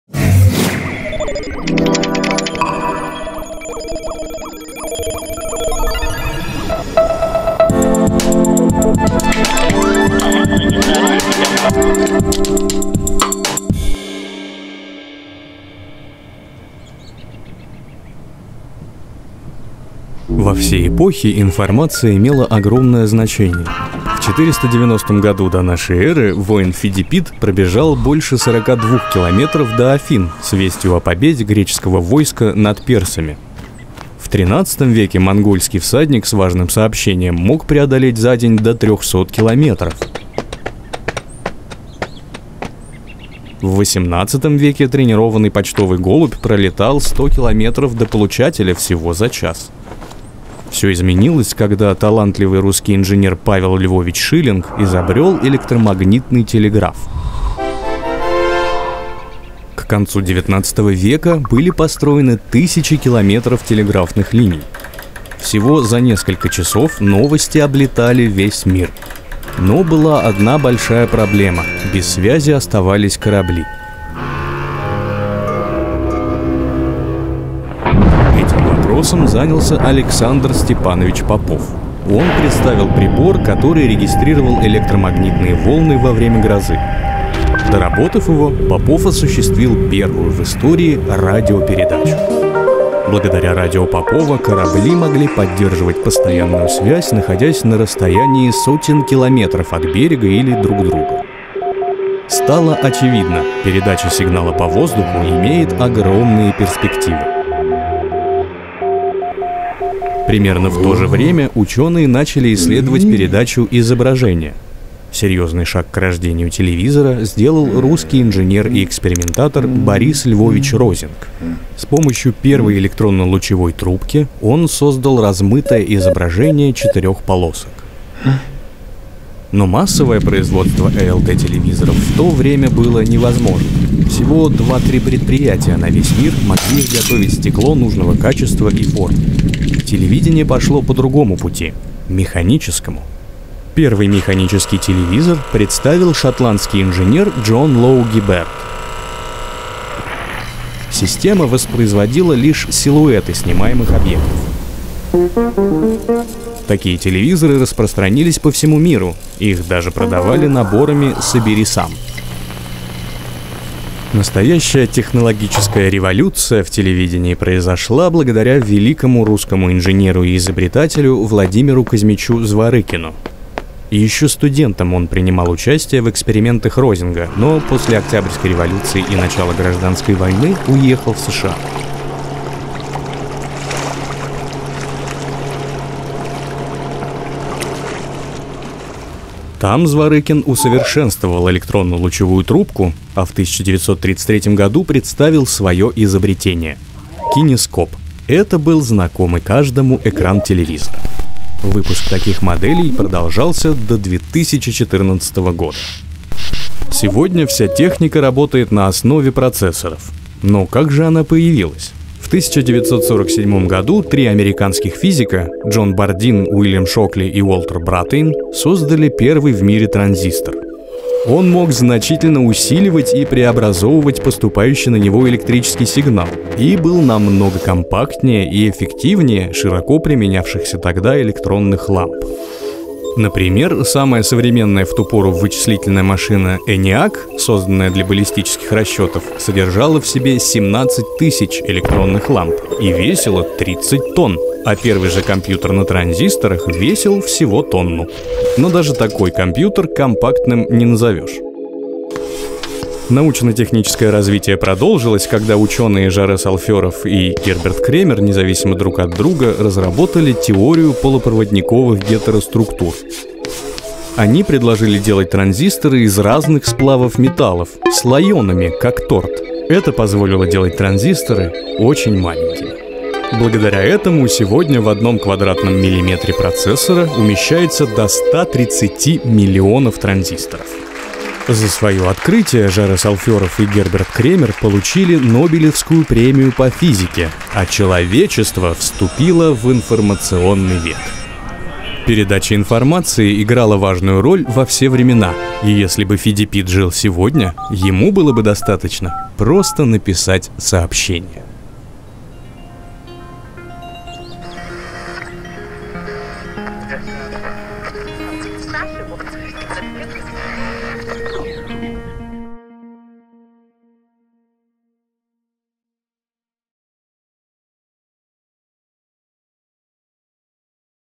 История радиоэлектроники | История российской науки
На протяжении столетий время доставки сообщения до конечного получателя зависела от скорости и выносливости человека, лошади или почтовой птицы. Но всё изменилось, когда в начале XIX века русский инженер Павел Львович Шиллинг изобрел первый электромагнитный телеграф. В этой серии вы узнаете, как российские и советские ученые повлияли на развитие радио, телевидения и современной электроники.

Серия выходит в рамках анимационного проекта "История российской науки", в котором мы познакомим вас с тем, какой вклад российские ученые сделали в мировую науку.